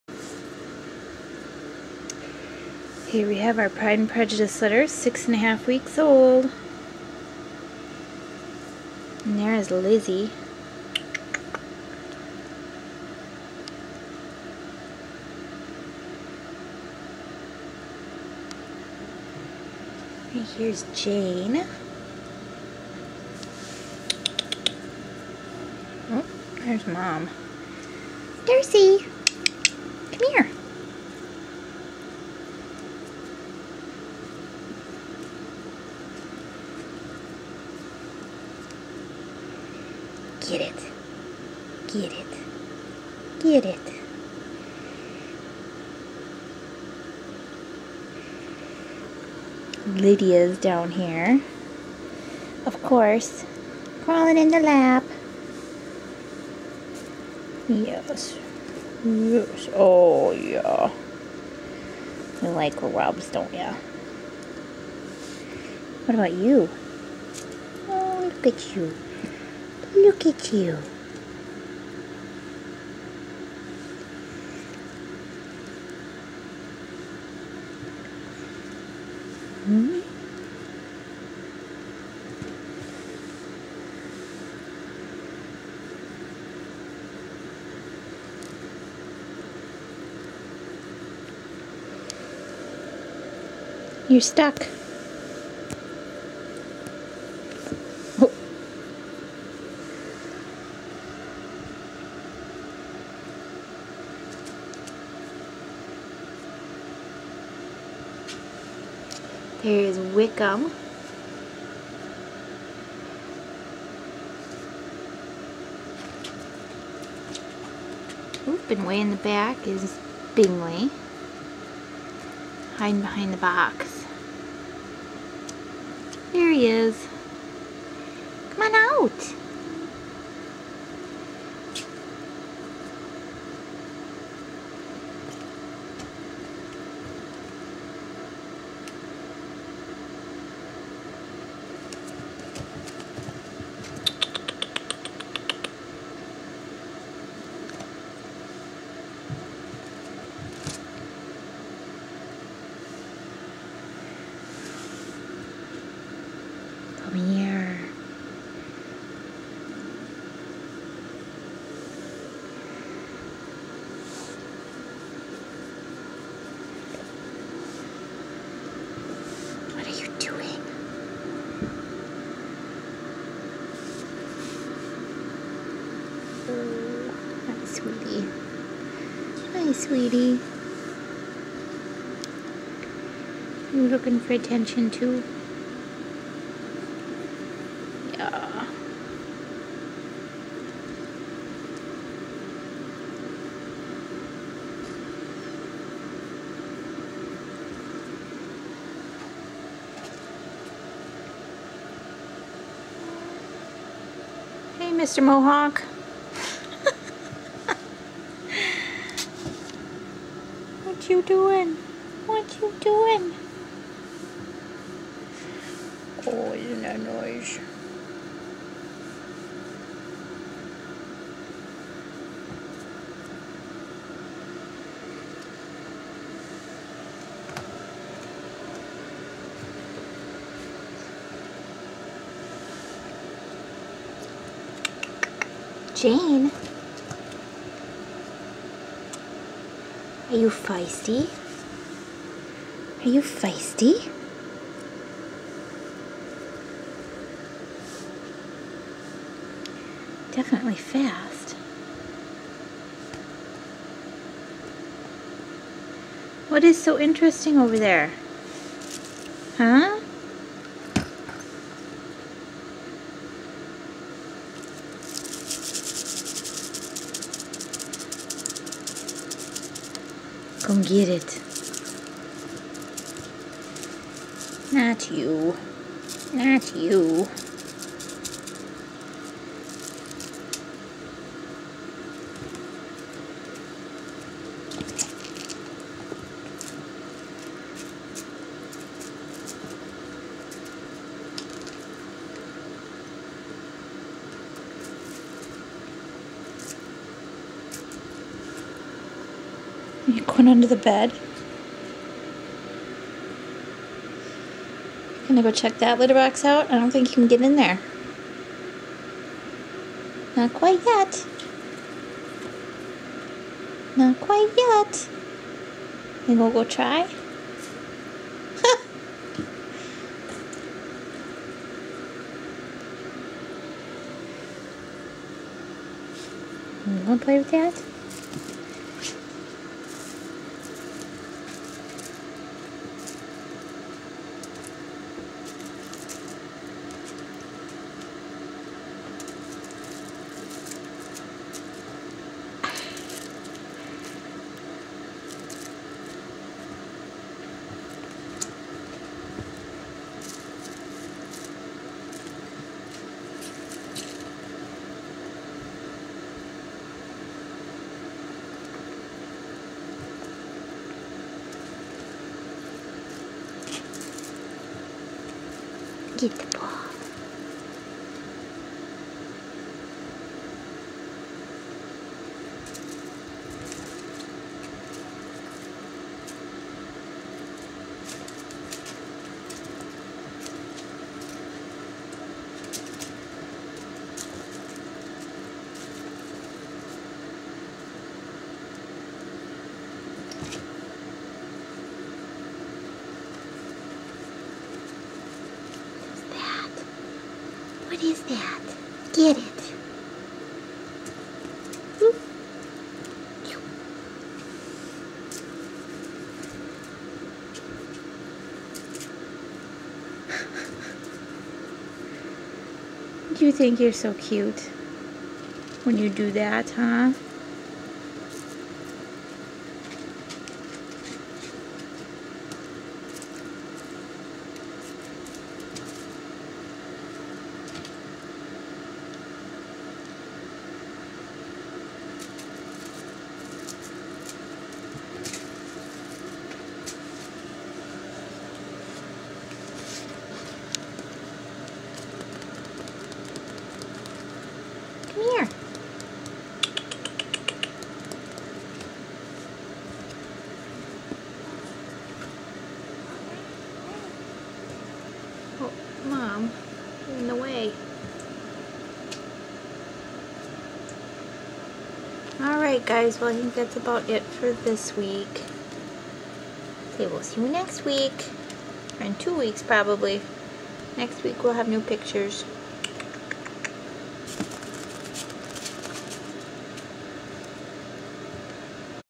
RagaMuffin Kittens 6 1/2 weeks-Imperial Rags
Hi!  Here is the P&P litter at 6 1/2 weeks.